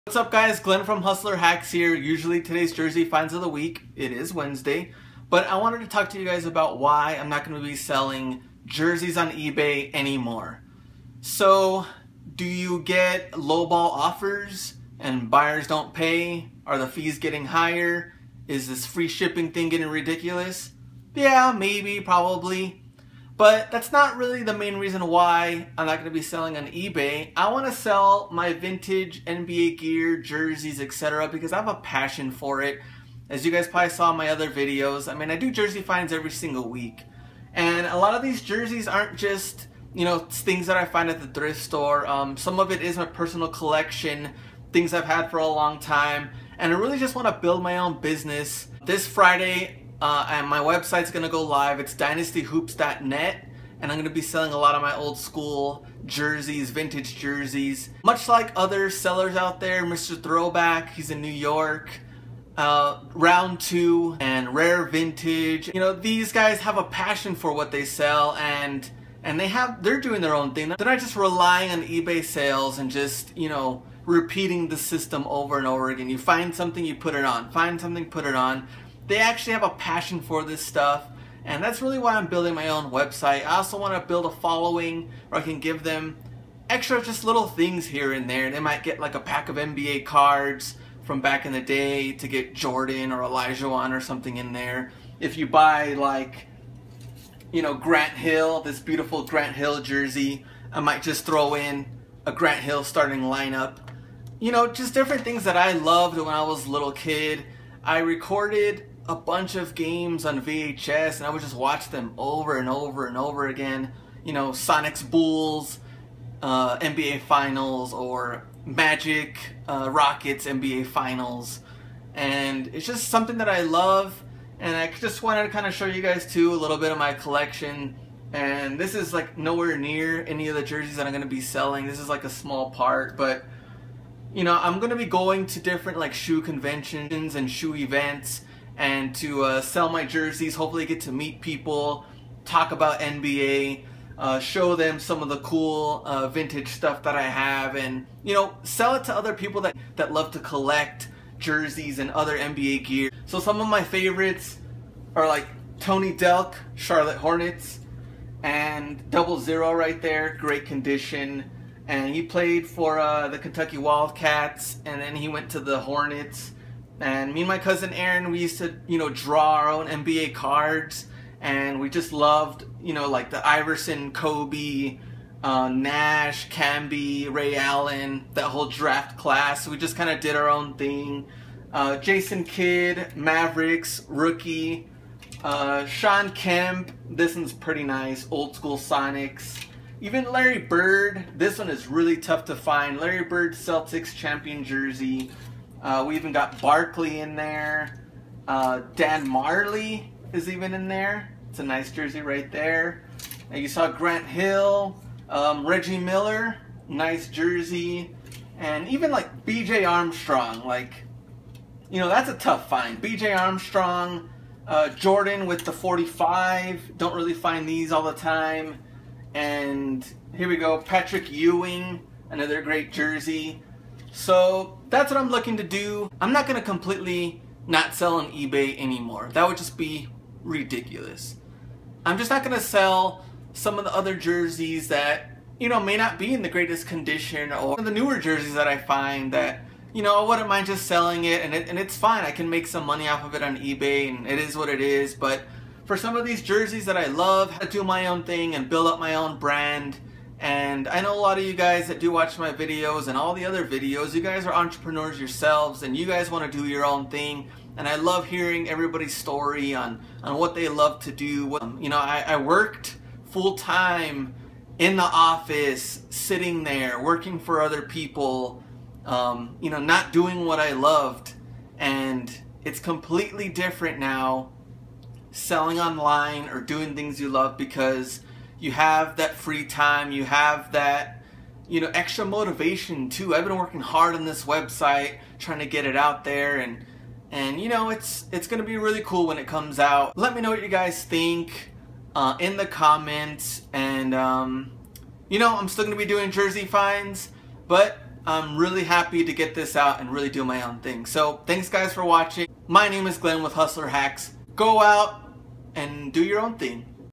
Why I am NOT selling jerseys on eBay, Creating my own website
Glen tells you why he is creating his own website for jerseys.